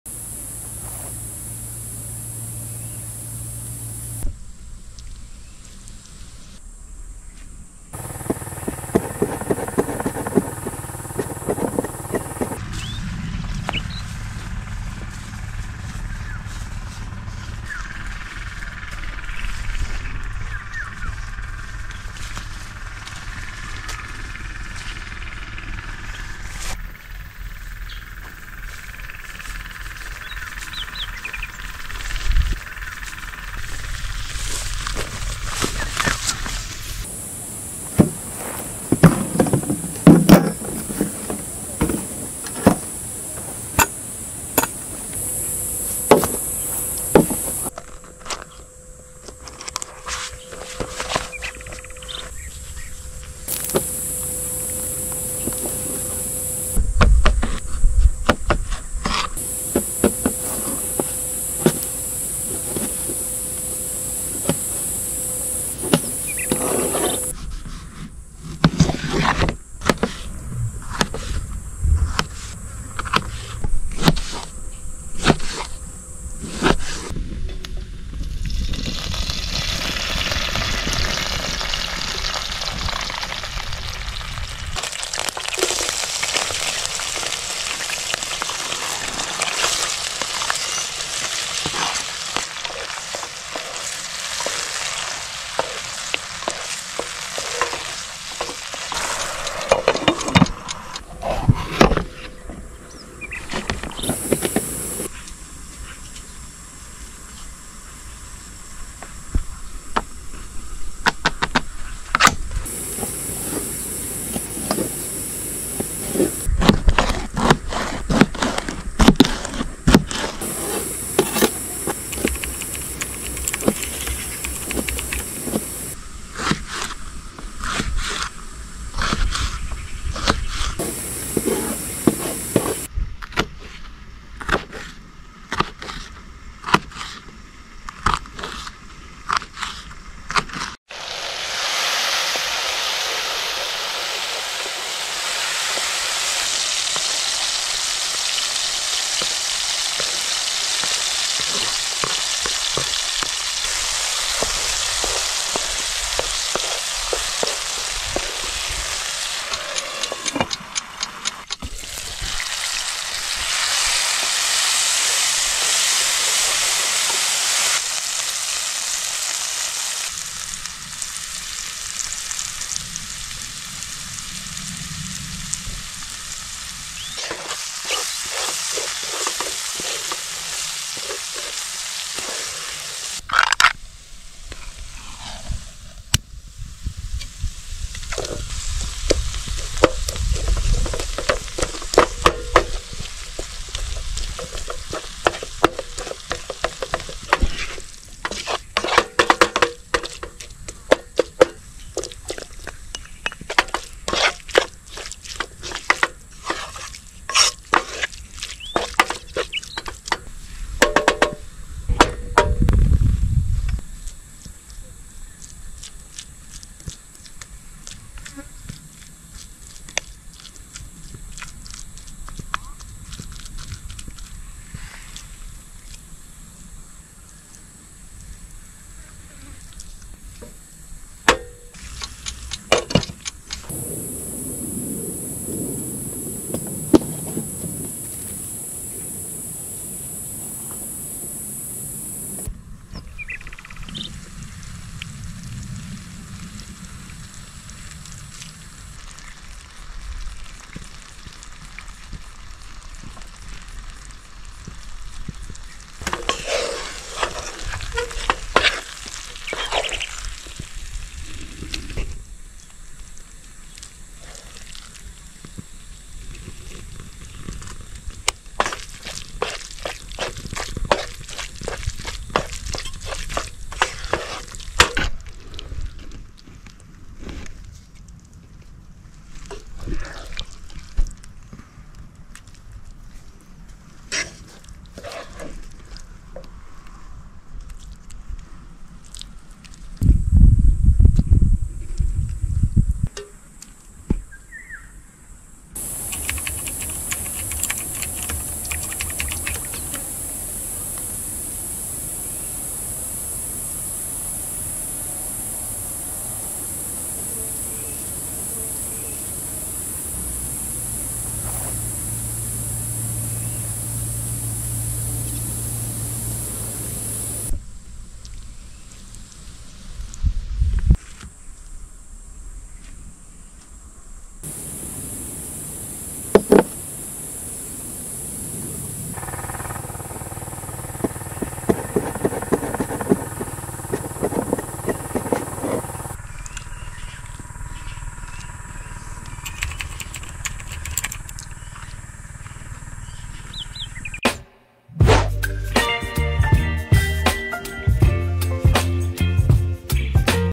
The most delicious snack you have ever eaten with tuna. I cooked this simple food in nature
I hope you like this recipe make it and enjoy!
What food from your country or city would you like me to cook?